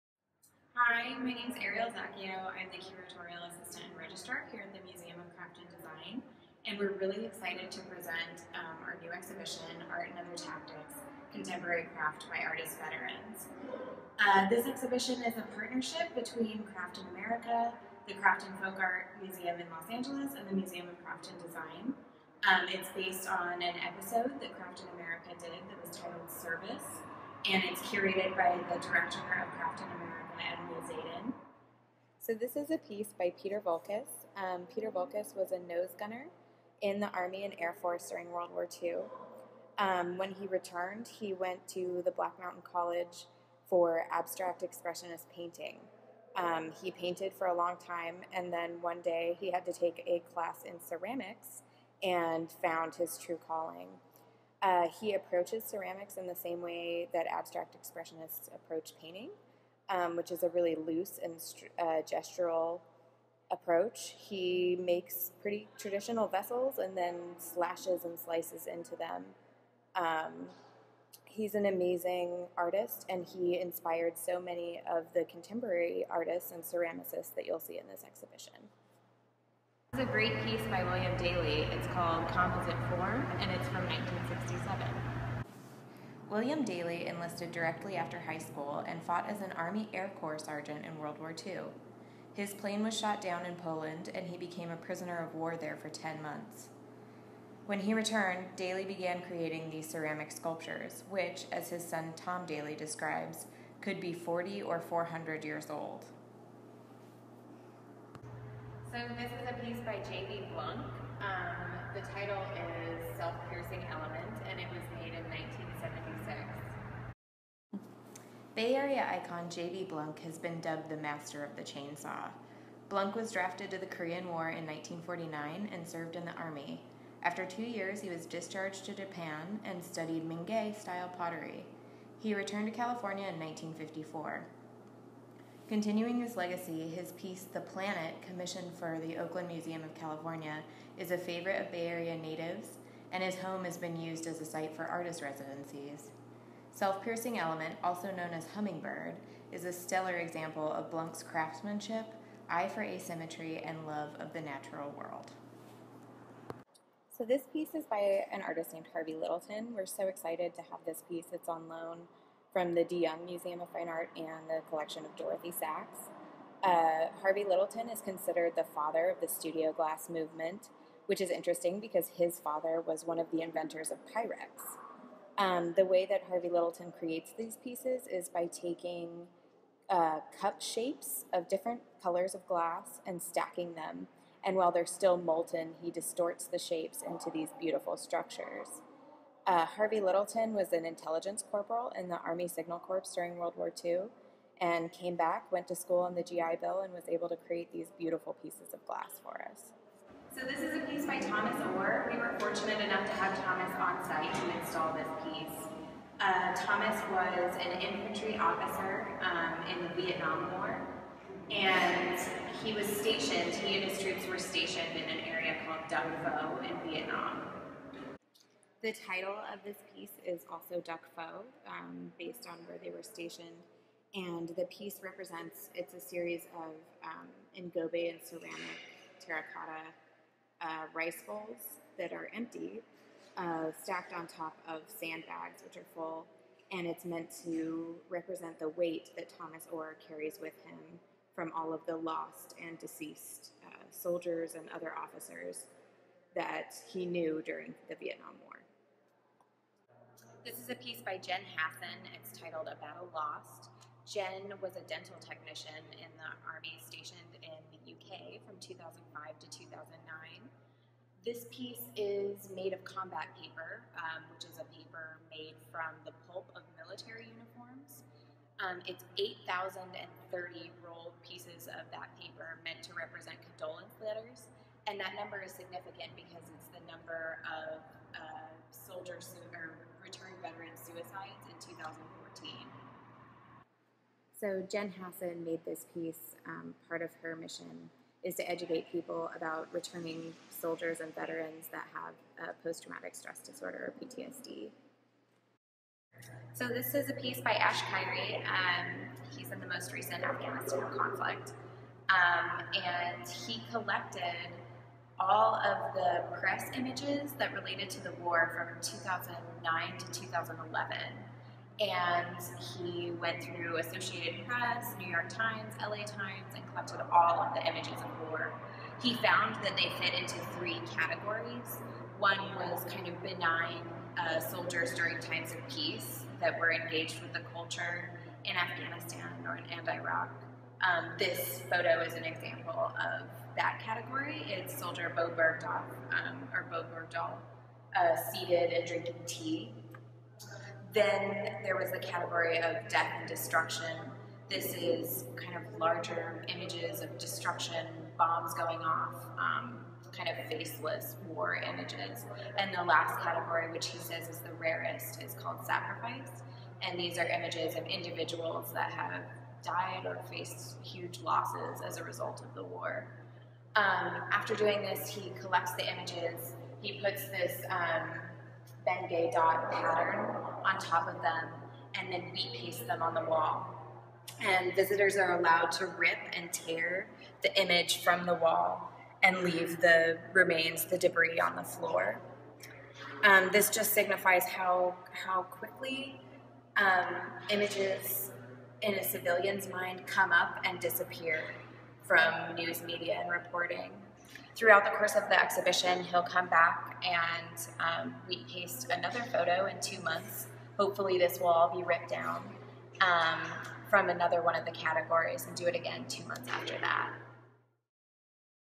Art and Other Tactics: Contemporary Craft by Artist Veterans
Enjoy this video walkthrough with Curatorial Assistant / Registrar Ariel Zaccheo at the Museum of Craft and Design in San Francisco.  "Art and Other Tactics: Contemporary Craft by Artist Veterans" is on view through March 20, 2016.  Visit SFMCD.org for more details.

Featured works in order of appearance:
- "Stack" by Peter Voulkos, 1990. Clay.
- "Composite Form" by William Daley, 1967. Unglazed stoneware.
- "Self-Piercing Element" by JB Blunk, 1976. Redwood
- "300o Rotated Ellipsoid" by Harvey Littleton, 1980.  Barium-potash glass with multiple cased overlays of Kugler color.
- "Duc Pho" by Thomas Orr, 2013. Ceramic, sandbags, canvas.
- "A Battle Lost" by Jen Hassin, 2015. Rolled condolence letters, paper made from military uniforms, gold foil, and matte medium.
- "Untitled Media Images" by Ash Kyrie, 2015.  Copy Paper and wheat paste.

Gallery installation photos by Kurt Lundquist.